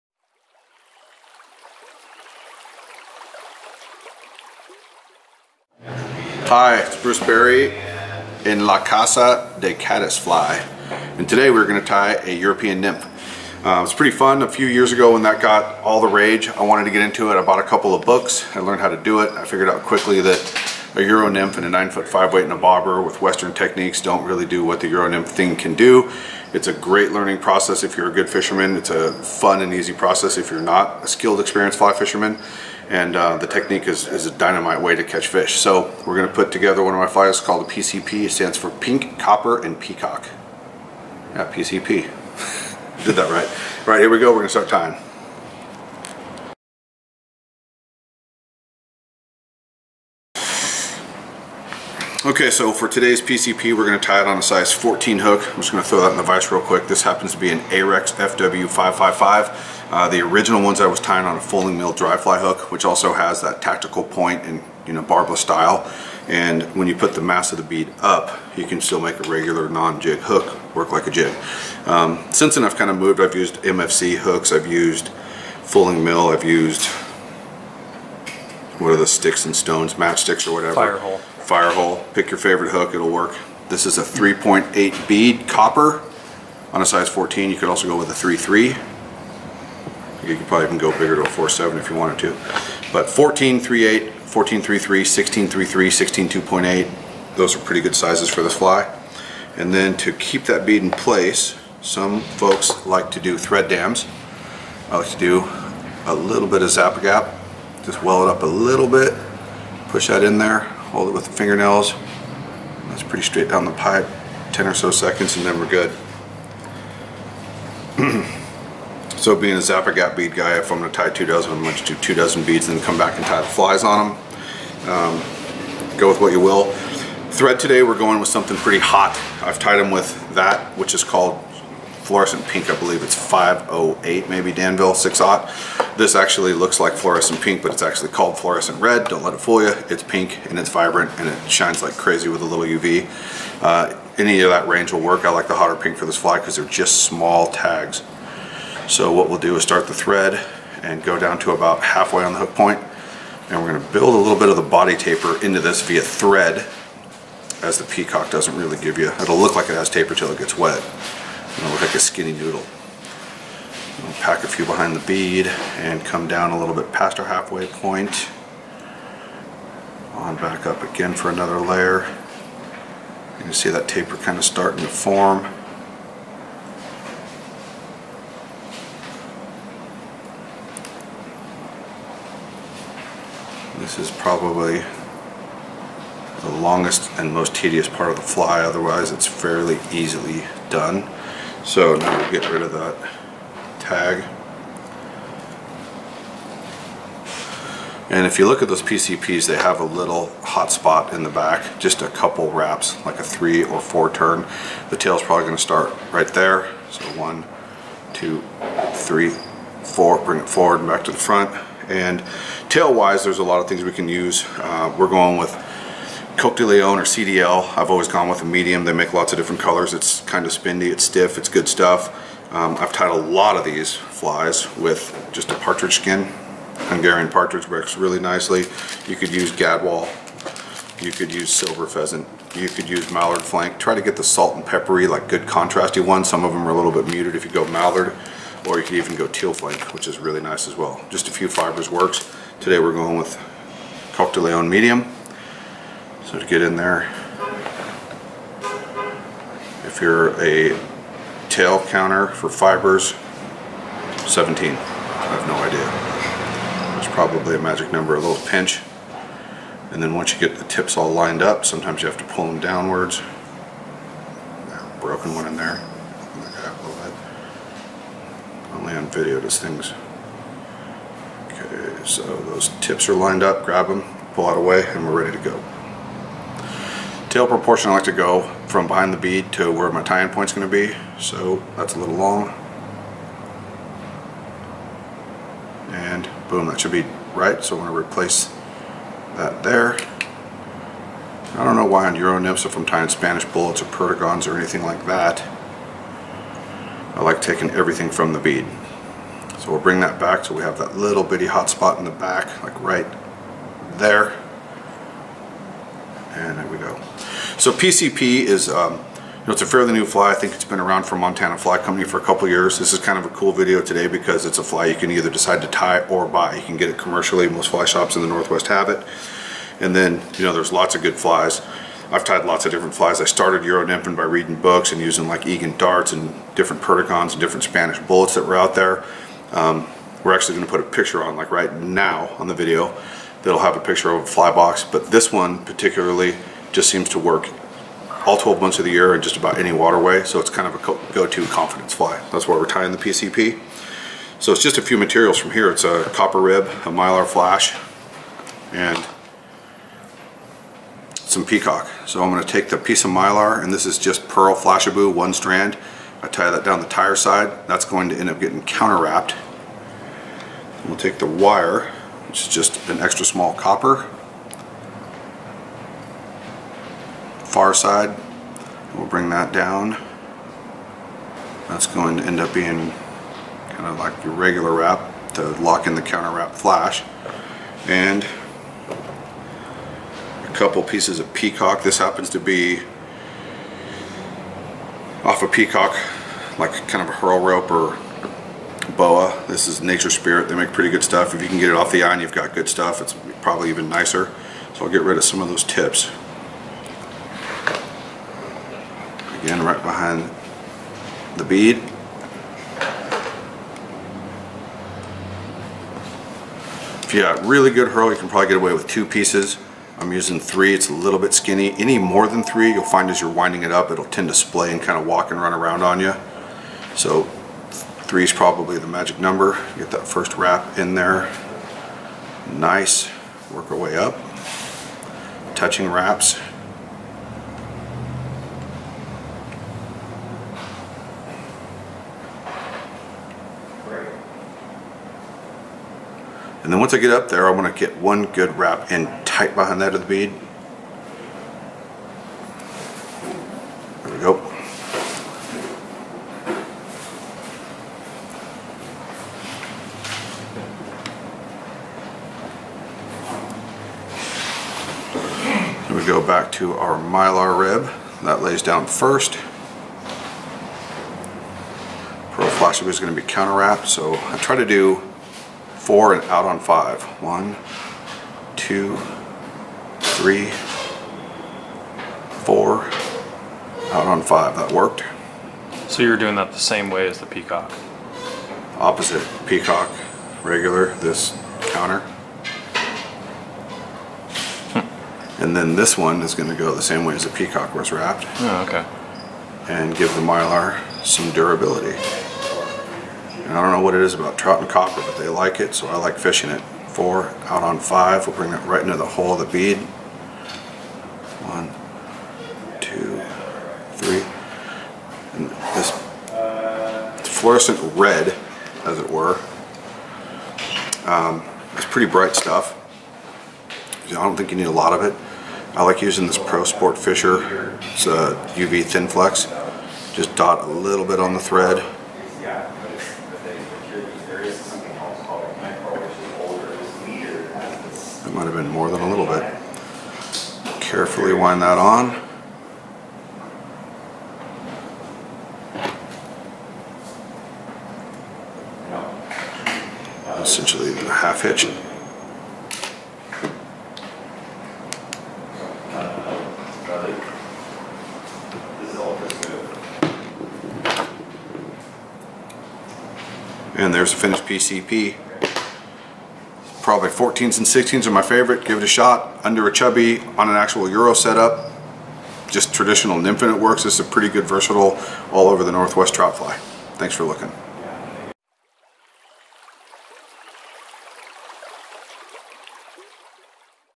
Bruce Berry's Pink, Copper, & Peacock "PCP" Jigged Euro Nymph Fly Tying Instructional Video
In this video, Bruce Berry joins us at the Caddis Fly shop to tie a favorite staple of his jig nymph lineup, the PCP. The fly is simple and deadly, and deserve a spot in any angler's nymph lineup!
Materials List:
Hook: Ahrex FW555, Sz14
Bead: 3.8mm Copper Slotted Tungsten Bead
Thread: Flo. Red Danville 70D
Tail: Coq De Leon
Body: Peacock Herl
Rib: Micro Flashabou  Copper Wire, small
Glue: Zap-a-gap, Pro Sportfisher UV Thin Flex Clear